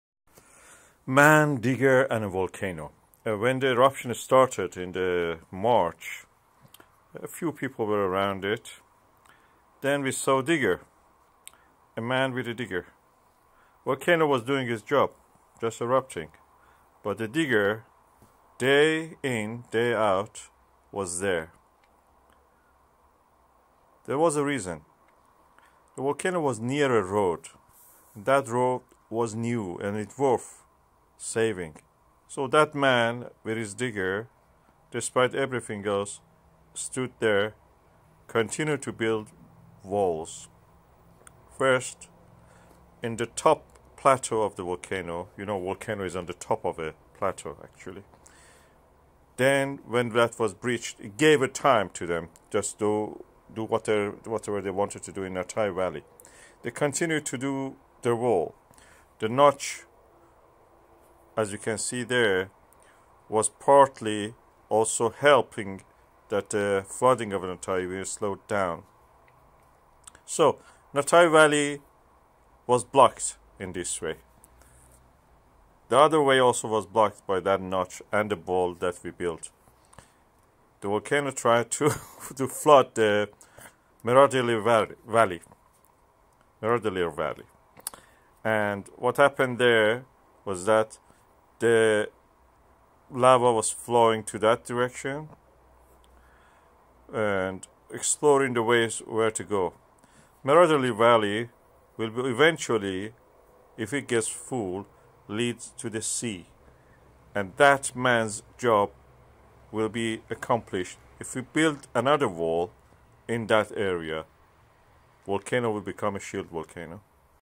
How A Man With Digger Stopped A Volcano/ Meradalir/ Iceland Fagradalsfjall Geldingadalir Volcano
LANDSAT-8 satellite image from NASA & USGS, showing SW Iceland and the eruption site - both as a true colour composite and with shortwave/near infrared light. That way, the active areas can more easily be detected.
Image credit:

Nature and Travel | Iceland Monitor | Mon 28 Jun 2021 | 11.00 GMT

A satellite picture taken Saturday shows much more volcanic activity than before at the eruption site by Fagradalsfjall, Southwest Iceland, mbl.is reports. If this development continues, lava will flow sooner than previously expected from Nátthagakriki valley over Suðurstrandarvegur road - an important road that connects the town of Grindavík and South Iceland.

The picture, taken using near infrared light, shows heat distribution in the lava, where white areas indicate the vent and lava channels, and active lava flow is red and yellow.